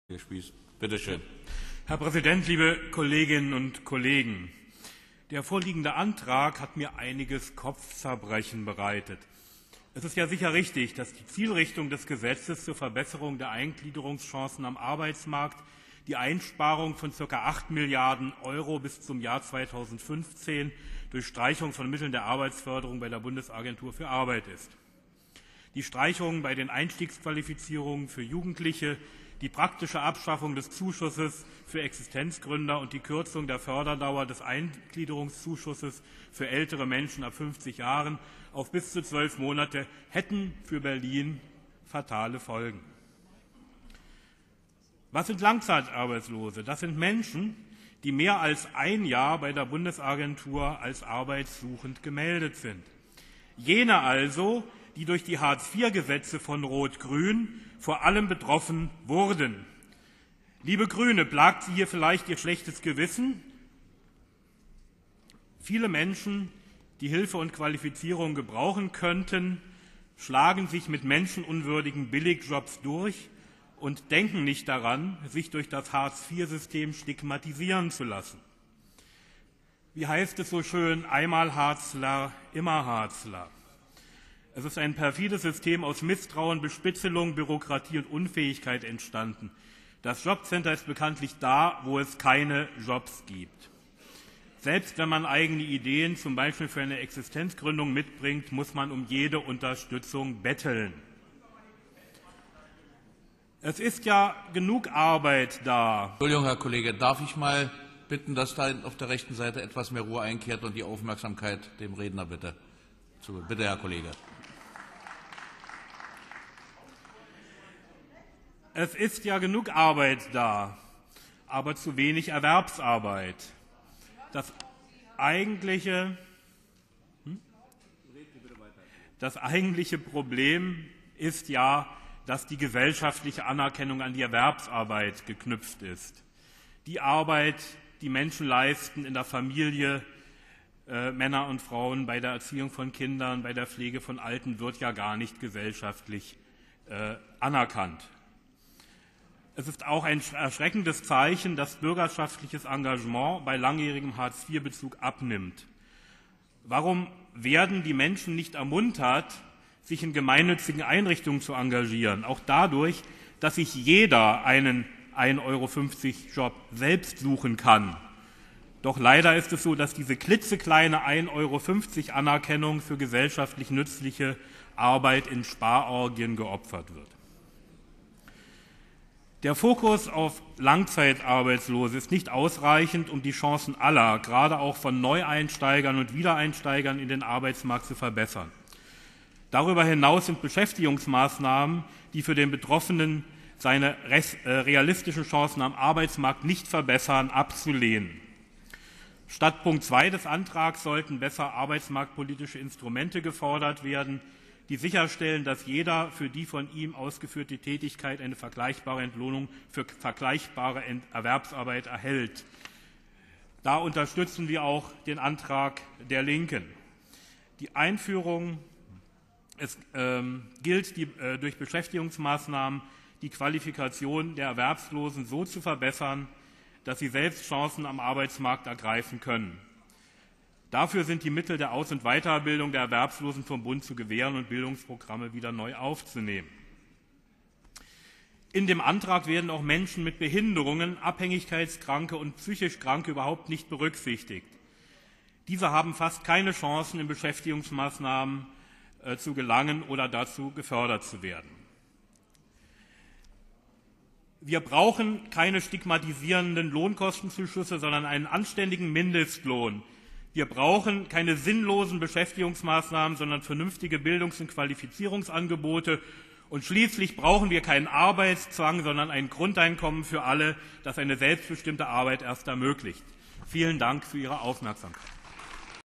10.11.2011 - Abgeordnetenhaus Berlin - Chancen für Langszeiterwerbslose - Alexander Spies (Piraten)
Chancen für Langszeiterwerbslose

Hintergrund für den Entschließungsantrag Bündnis 90/Die Grünen ist das "Gesetz zur Verbesserung der Eingliederung am Arbeitsmarkt", das die Bundesregierung im September beschlossen hatte. Es werde den "Interessen arbeitsuchender Menschen und vor allem dem dringenden Ziel der Bekämpfung der Langzeiterwerbslosigkeit in keiner Weise gerecht", heißt es in dem Antrag. Dieser und ein Änderungsantrag der Linken werden in die Ausschüsse verwiesen.

Alexander Spies (Piraten) plädiert für einen Mindestlohn, für "vernünftige Bildungs- und Qualifizierungsangebote" und ein Grundeinkommen für Arbeitslose. Hartz IV habe ein "perfides System aus Misstrauen, Bespitzelung, Bürokratie und Unfähigkeit" hervorgebracht.

10.November 2011
2. Sitzung des Berliner Abgeordnetenhauses